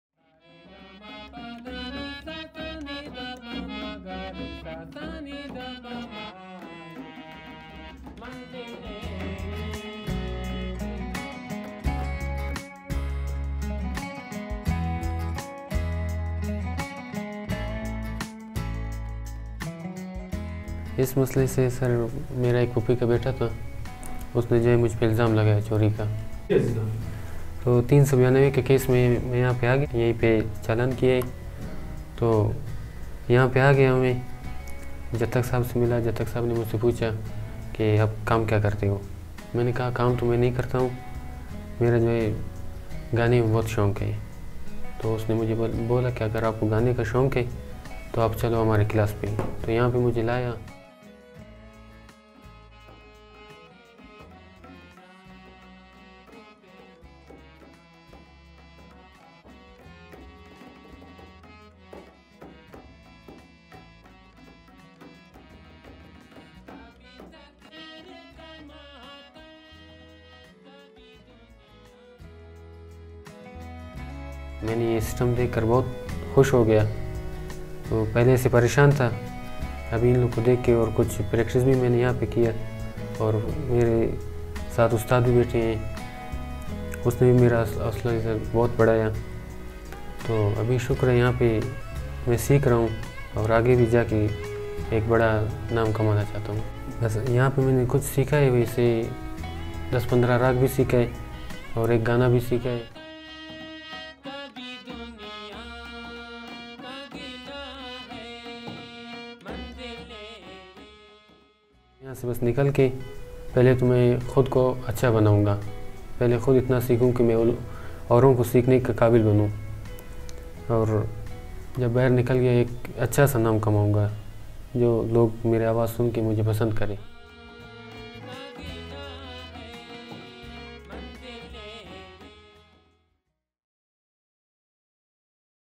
Prisoners taking music classes in Quetta Jail
فیصل آباد میں چھائے گڑ میلے کے رنگ